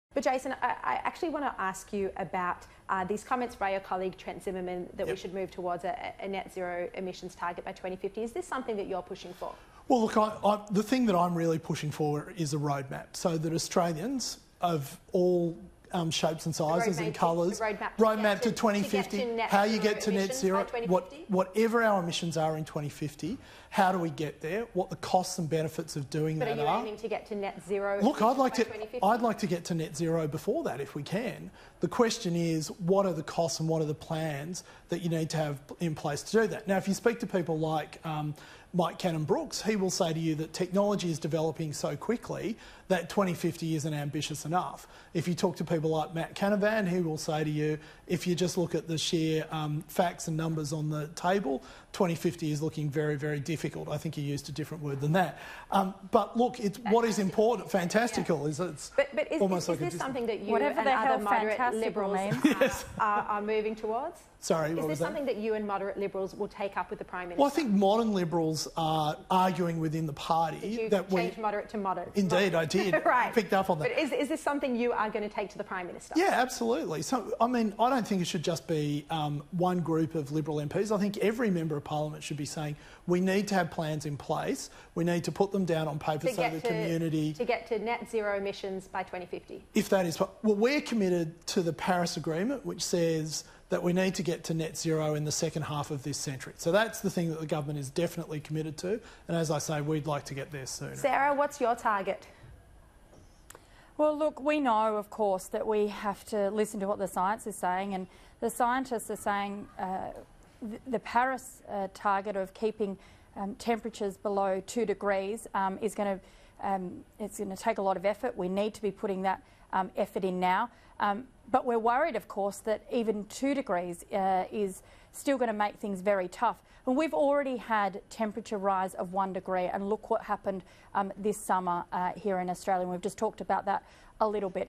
Jason Falinski wants to ‘get to net zero emissions before 2050’
Liberal MP Jason Falinski says “modern Liberals” are pushing for a net zero emissions target by 2050 in a move set to divide the party room.

Fellow ‘moderate Liberal’ Trent Zimmerman has argued for the government to adopt a target of zero emissions by 2050, but Mr Falinski said he would “like to get to net zero before that”.

Mr Falinski told Sky News he is “pushing for a road map to 2050” which will outline how the government can “get to net zero”.